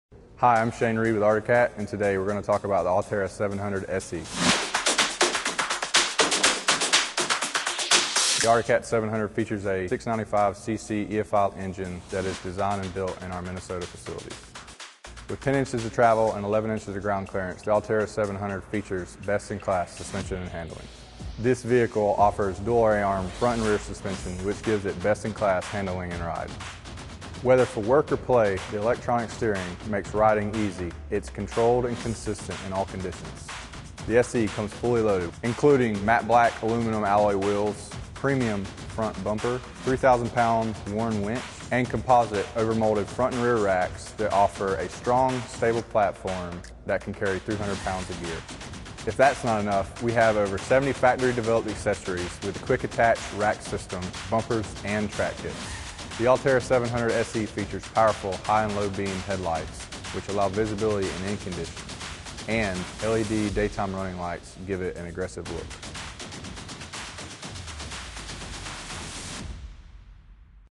Arctic Cat Alterra 700 SE ATV
ALTERRA 700: Ride First Class. A premium ATV with a smooth ride, a sleek design and unmatched power. Available at The Cat Shack Peterborough.